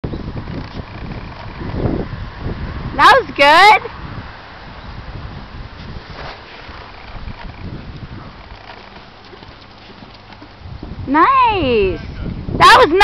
Trying out Inconclusive
Trying out a horse about a month ago. I was maybe going to lease this horse while Carty's recovering..but i decided to just take a small break from riding instead